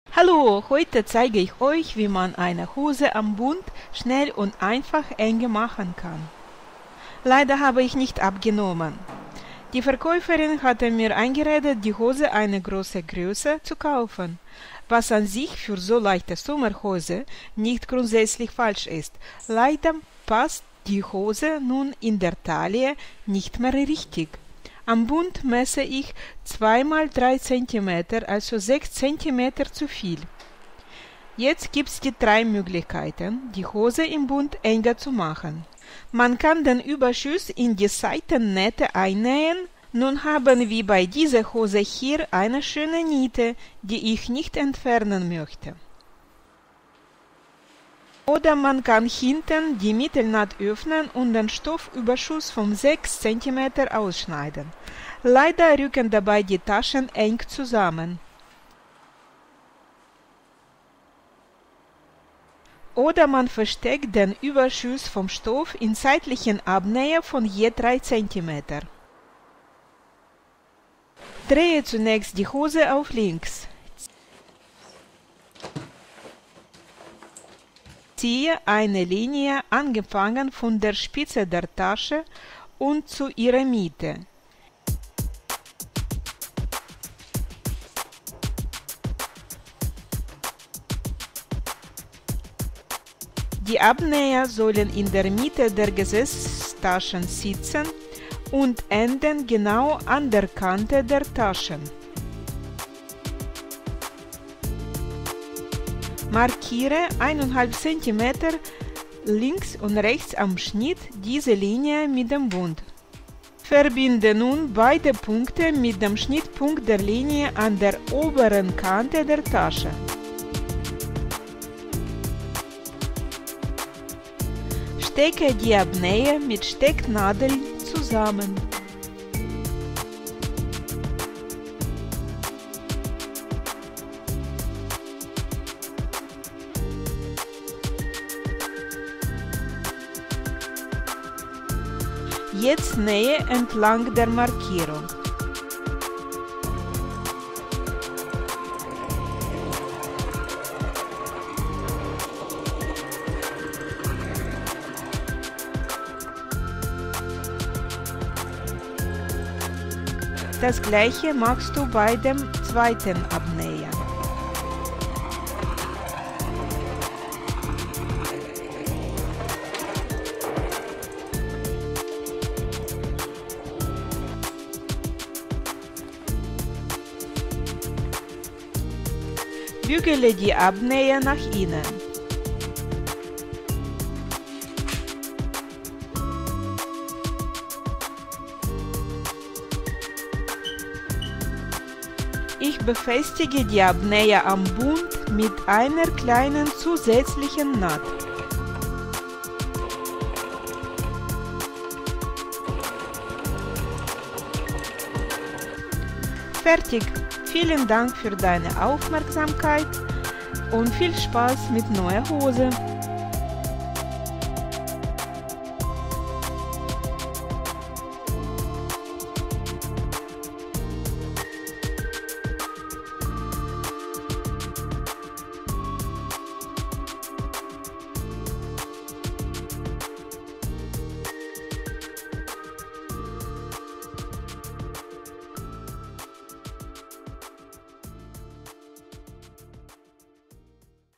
Hose am Bund enger machen in 10 Min.: schnell und einfach Abnäher setzen
Hose sitzt in der Taille nicht richtig.
Am Bund messe ich 2x3cm=6 cm zu viel. Jetzt gibt es drei Möglichkeiten, die Hose im Bund enger zu machen:
- man kann den Überschuss in die Seitennähte einnähen, nur haben wir bei diese Hose hier eine schöne Niete, die ich nicht entfernen möchte.
- oder man kann hinten die Mittelnaht öffnen und den Stoffüberschuss von 6 cm ausschneiden, leider rücken dabei die Taschen eng zusamen;
- oder man versteckt den Überschuss in seitlichen Abnähern von je 3 cm.